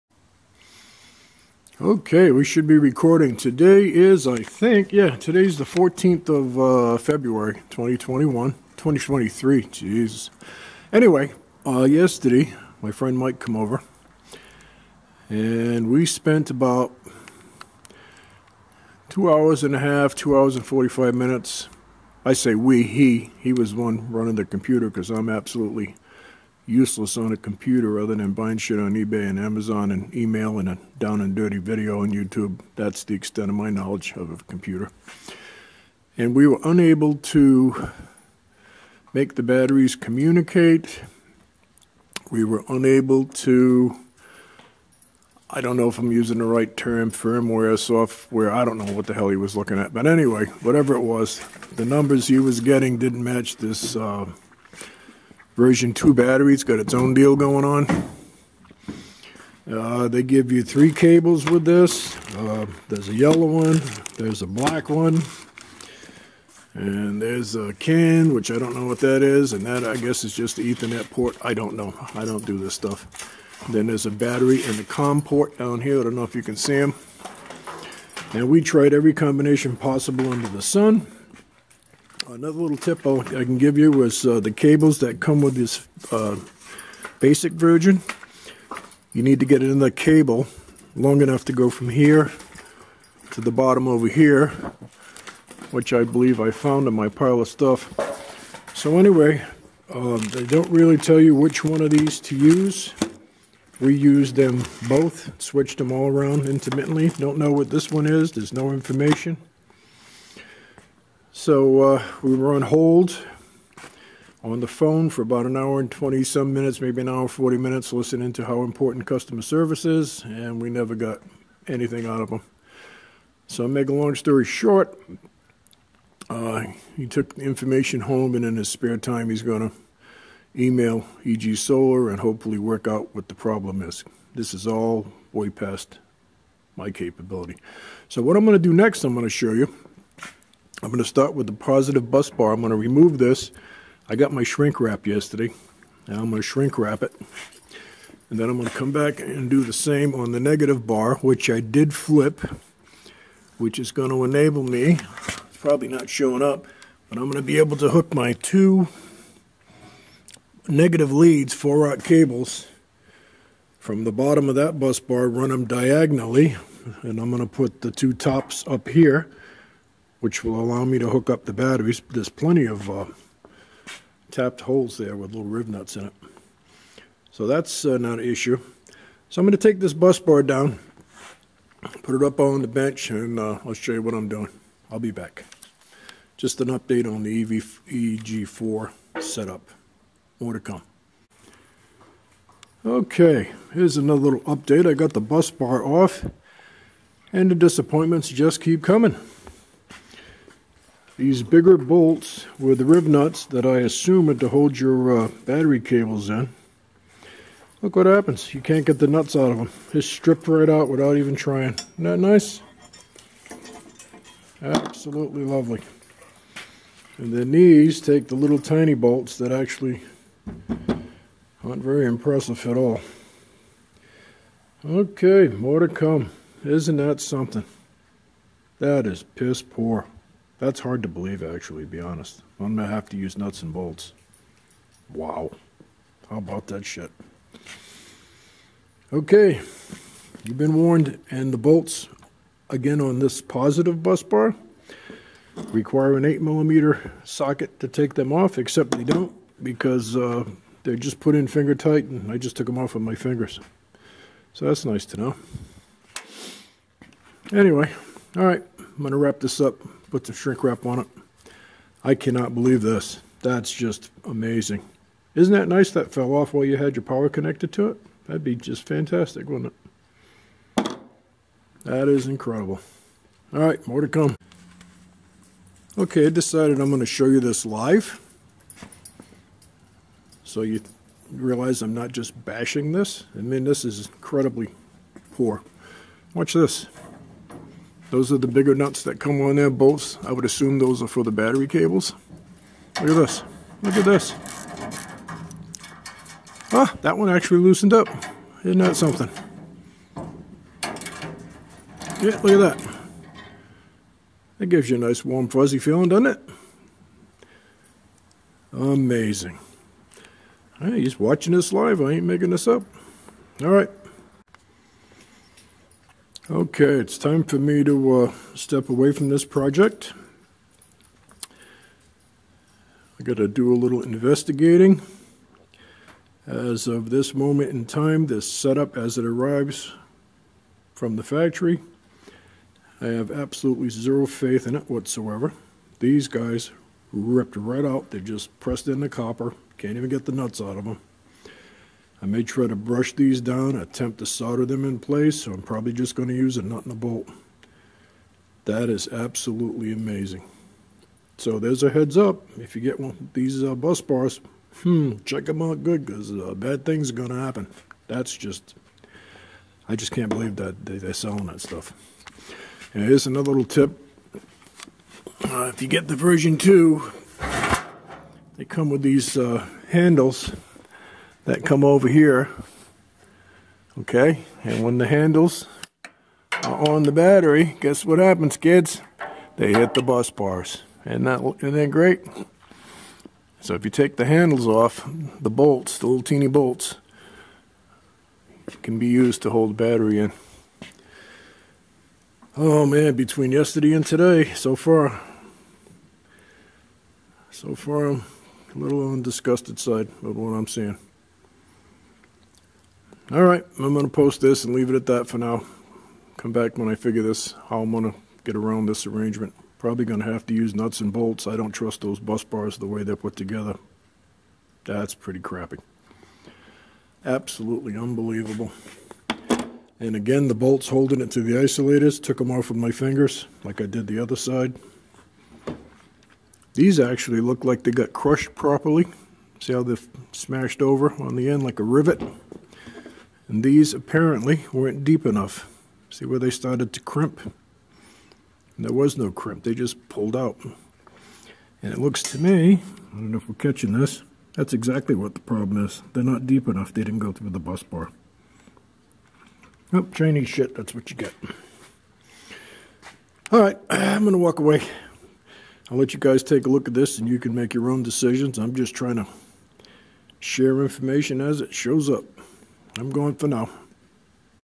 EG4 Battery & Bus Bar Info # 4 Unbelievable Safety Issue!!
Here's what I got so far. Glad I didn't  wait on buying this setup to use as soon as it got here. So Far no luck with battery communication at all. If U got 1 of these free server racks like I did please watch this video . I will show how poorly the bolts to bus bar install is as I did i tried to remove the bigger bolts.  This was unbelievable it is obvious no one is looking at these things & making sure they are safe. U have to watch this to believe it. If I sound a bit disgusted I am. This is amazing to me they would build something so dangerous. What the hell only potentially up to 600 amps shorting out, no big deal right?  BE SAFE.